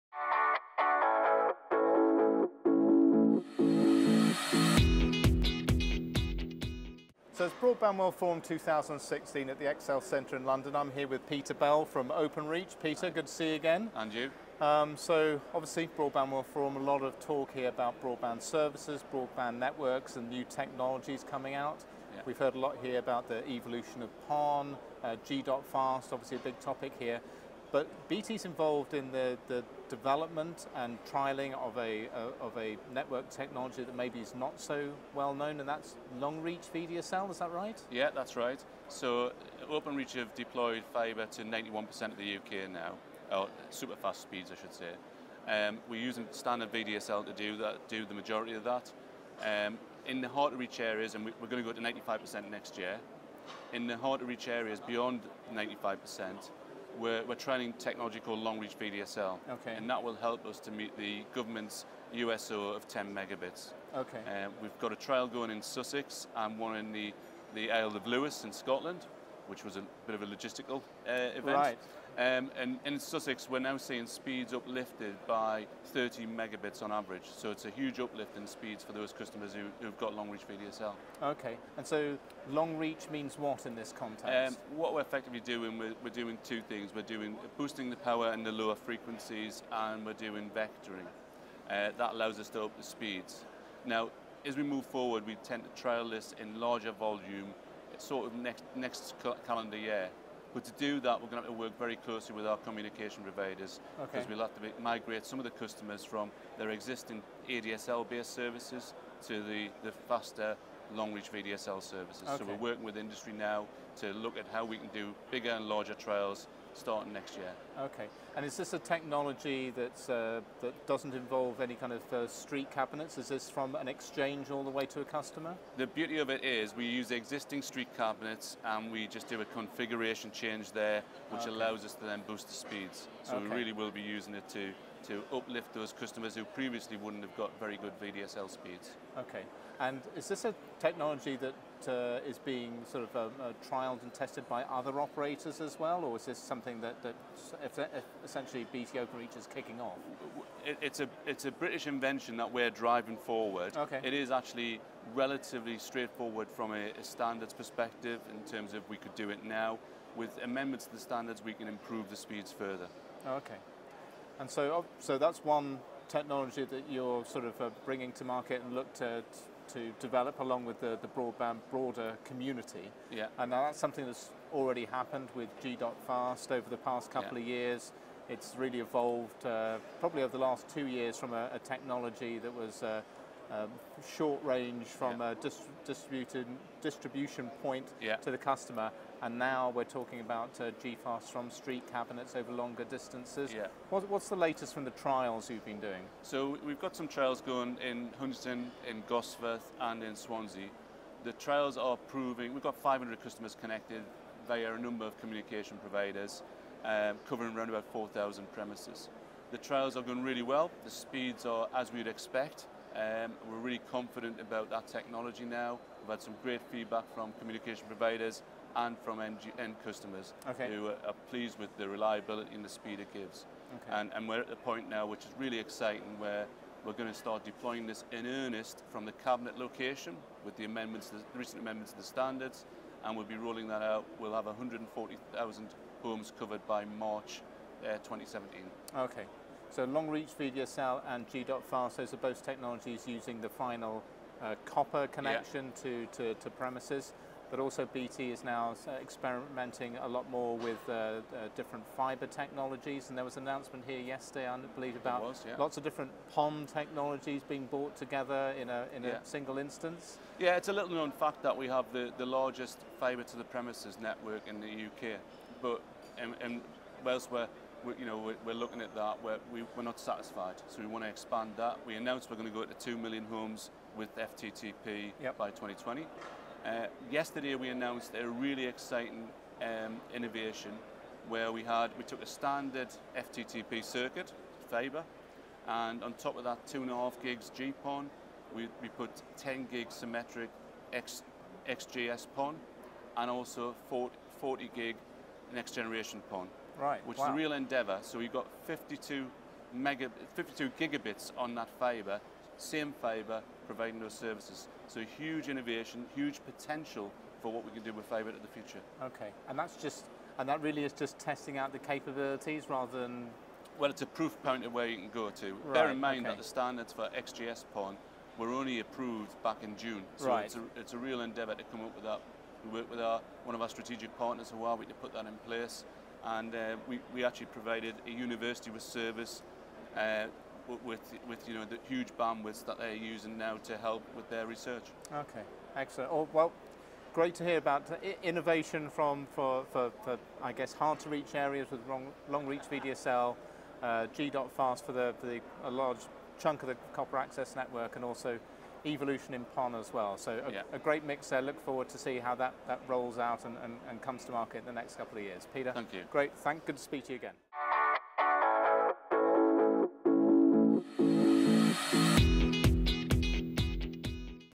Openreach to Deploy Long Reach VDSL2, G.fast and XGS-PON for Broader Coverage & Higher Bandwidth
In Britain, over 91% residential and enterprise users are enjoying 30 Mbit/s or higher bandwidth. Now, Openreach has been testing the new Long Reach VDSL2 technology and attempts to service 30 Mbit/s bandwidth for users in rural areas. It also trials and deploys G.fast at curb locations to provide 330 Mbit/s ultra-bandwidth for users. With the largest fiber to the premises (FTTP) network in Britain, Openreach is set to increase the number of FTTP users to 2 million by 2020.

At Broadband World Forum 2016, Openreach and Huawei jointly launched Europe's first FOA for TWDM PON, XGS-PON, and GPON transmission over the same fiber. This fiber can deliver a total capacity of 52.5 Gbit/s, meeting ultra-bandwidth requirements of University of Suffolk in their research. Thanks to these innovations, Openreach is going to provide better broadband services in Britain.